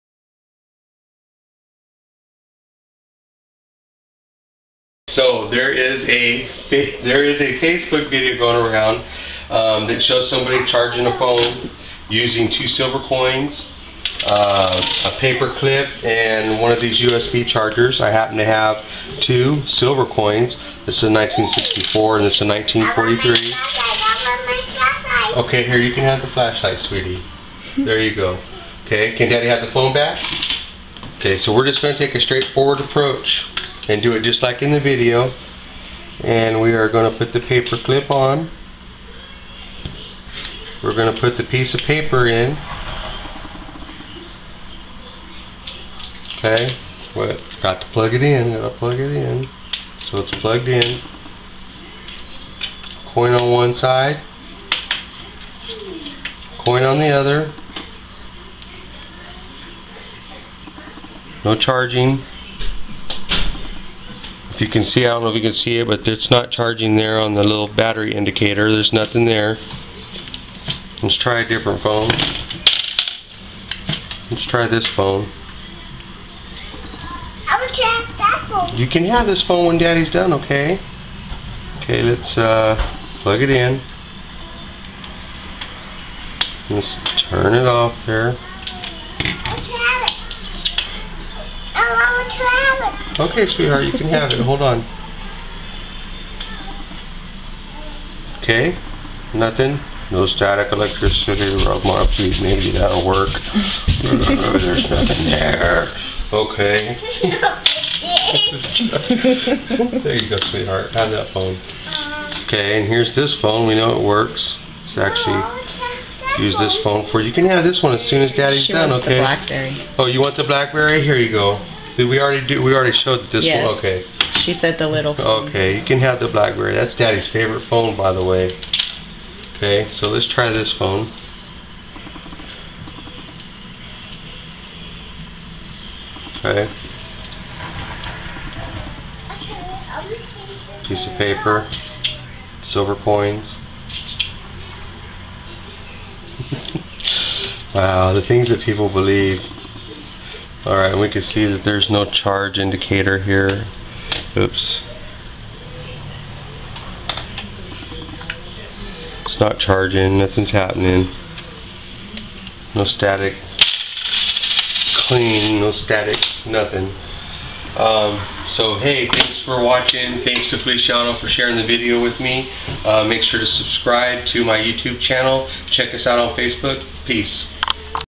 Can you charge a cell phone with body electricity!!?
Can you charge a phone with two silver coins? this is a video response to this video

  we put this viral Facebook video to the test